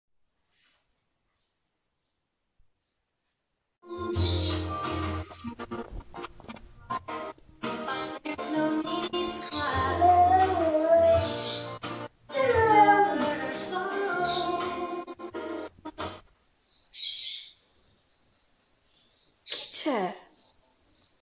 CUCCIOLO SAMOIEDO ULULA ALLA TV
RUNE SI RICONOSCE IN TV E RICONOSCE KIMBA CHE OGGI NON C'È PIÙ